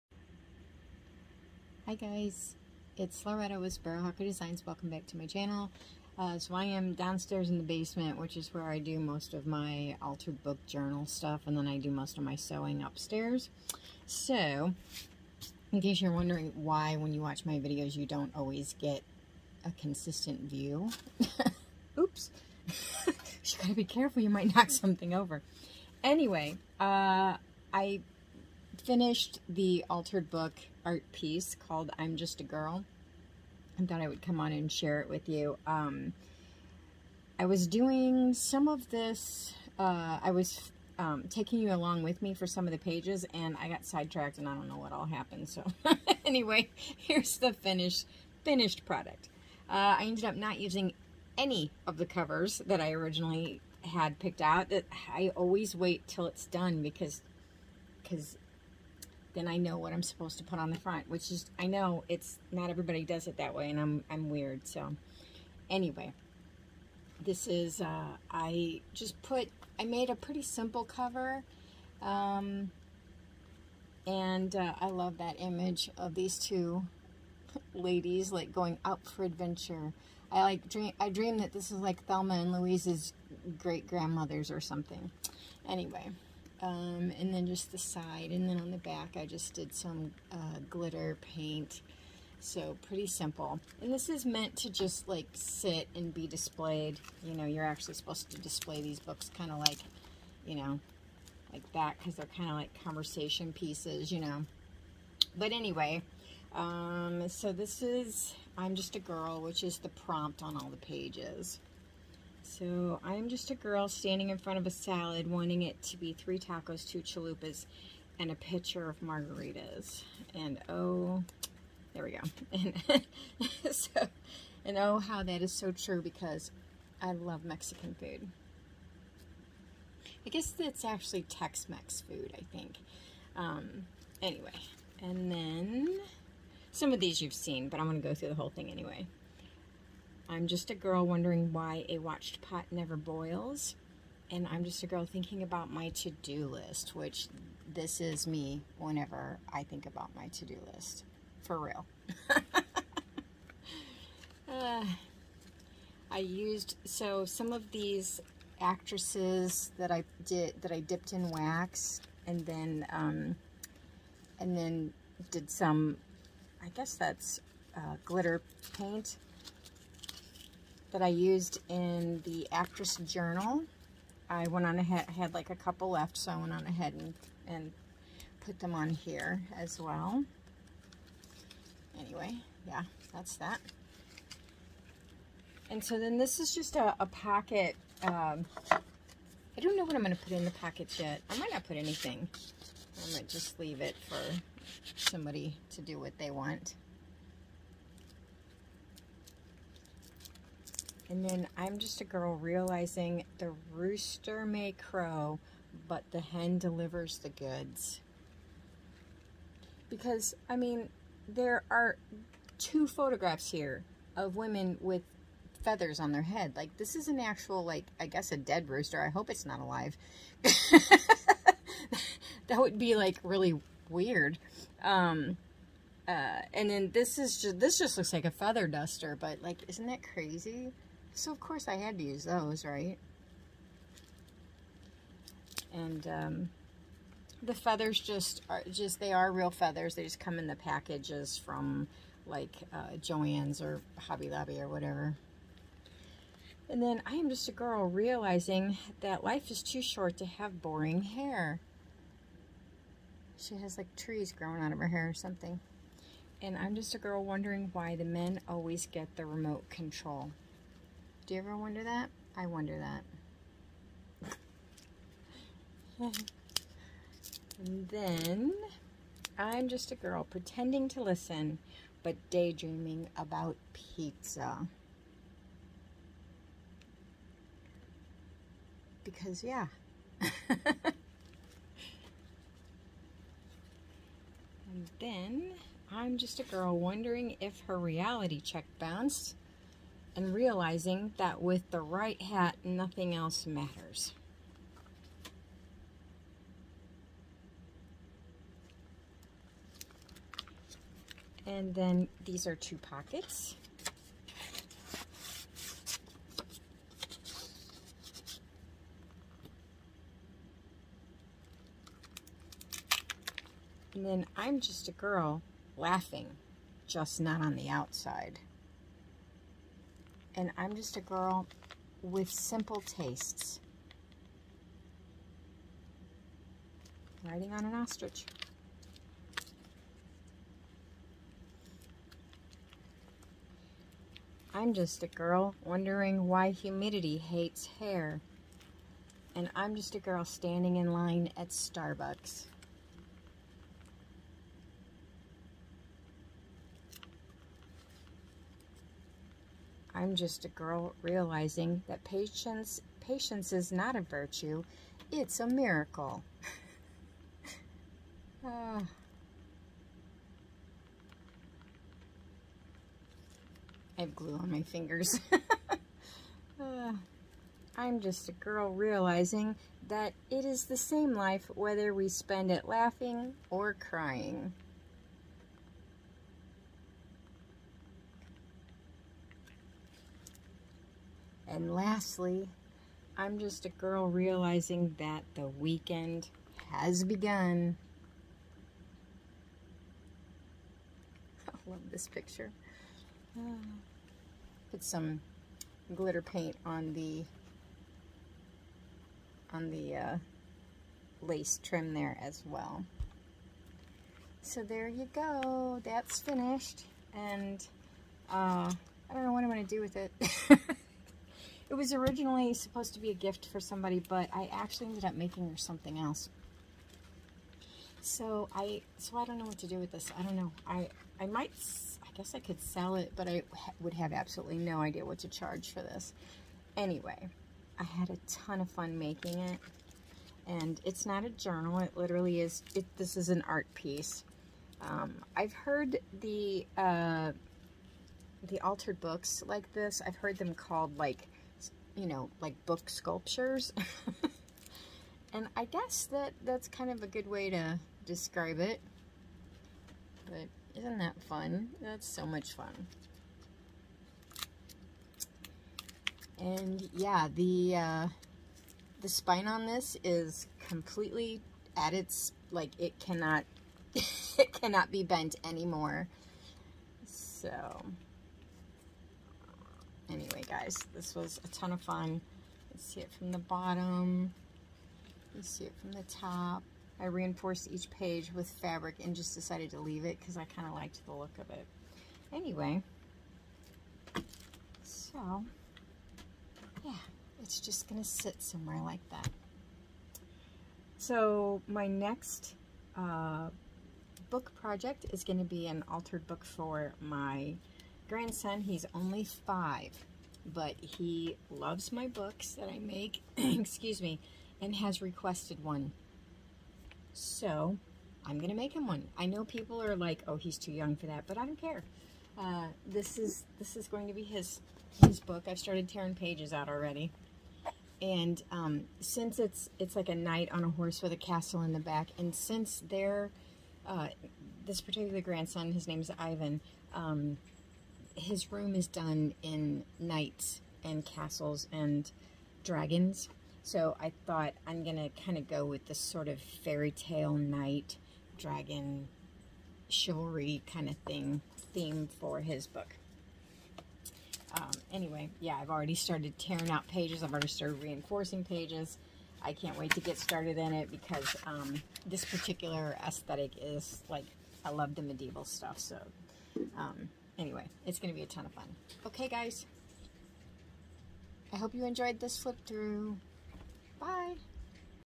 Altered Book (I'm Just a Girl) Final Flip Through
Hello lovely people! I have finished the I'm Just a Girl altered book and am taking you on a final flip through of the finished product. This was so much fun!!
This was a supposed to be a gift, but I made that person something else instead. So, I'm not sure what I'm going to do with it yet.
Thank you all for your YouTube love and your Etsy love.
Take care!
My etsy store